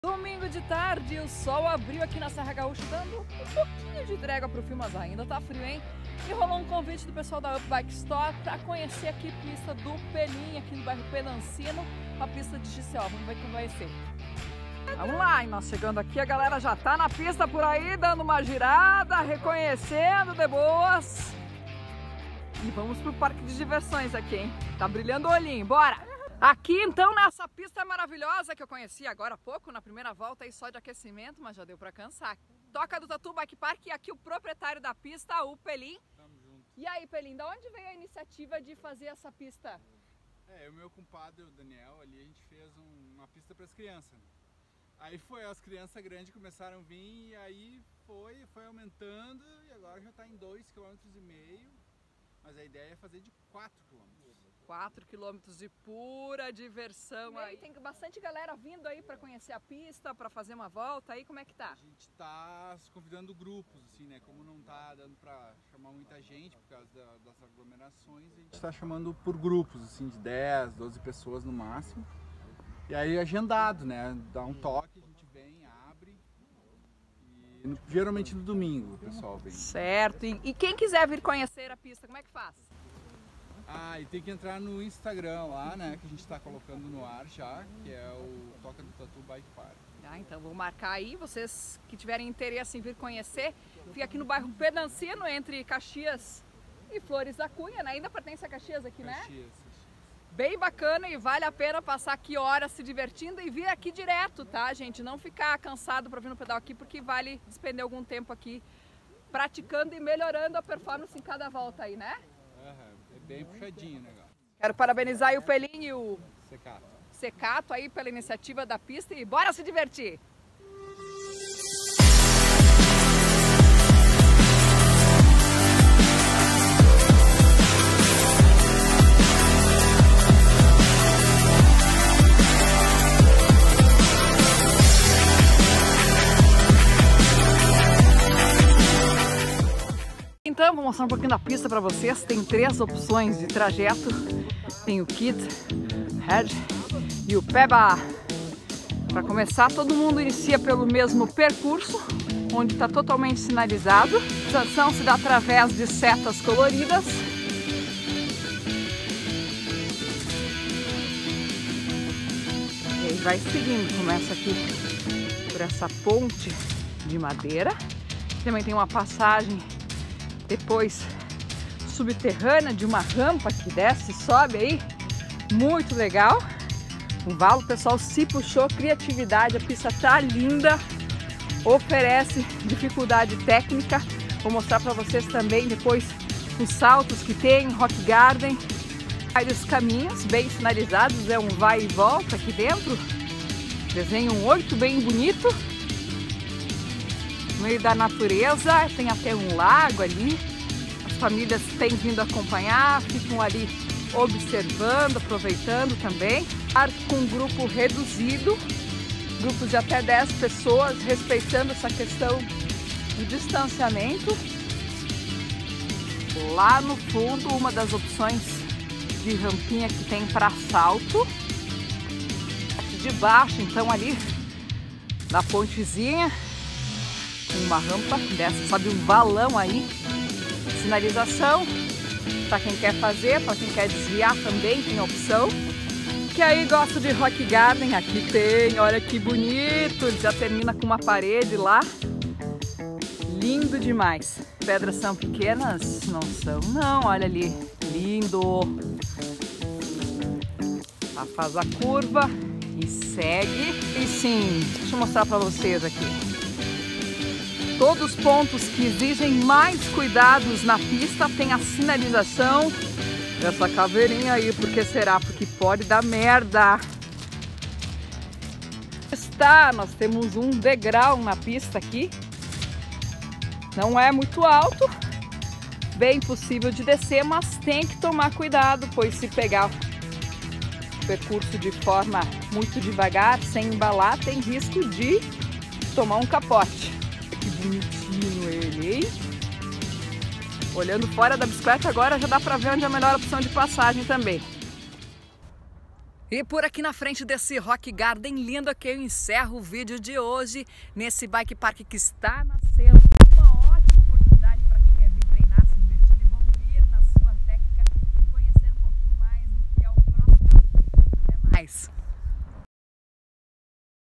Épika no Bike Park Toca do Tatu
Na sua cidade tem Bike Partk?
Tem Bike Park nascendo em Caxias do Sul! 🥳É Toca do Tatu Bike Park e eu @katiaweberassessoriaesportiva do Épika Bike  fui conferir, aproveitei para me divertir e gravar para vocês 🤩
Um circuito de XC com 3 opções de percurso totalmente sinalizado e com possibilidade de bastante treinamento de técnica no MTB.
Eu amei!
Quer conhecer? Agende no @tocadotatubikepark
Confira o vídeo!
Marque seus amigos que curtem trilhas😉